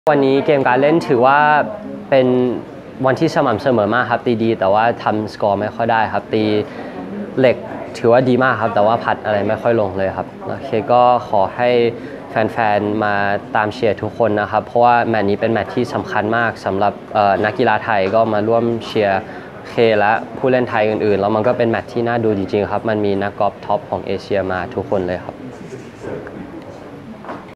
สัมภาษณ์ทีเค AAC รอบ 1
รัชชานนท์ AACGolf อมตะสปริงคันทรีคลับ เอเชียแปซิก "ทีเค" รัชชานนท์ ฉันทนานุวัฒน์ นักกอล์ฟหนุ่มน้อยวัย 15 ปี พูดถึงการเล่นในวันแรกเปิดกอล์ฟสมัครเล่น Asia-Pacific Amateur Championship ที่อมตะ สปริง คันทรี คลับ จังหวัดชลบุรี เมื่อวันพฤหัสบดีที่ 27 ตุลาคม 2565 ซึ่งทำ 5 เบอร์ไม่เสียโบกี้เลยสกอร์ 5 อันเดอร์พาร์ 67 รั้งอันดับ 2 ร่วมตามหลัง จิน ปั๋ว จากจีนผู้นำอยู่ 2 สโตรก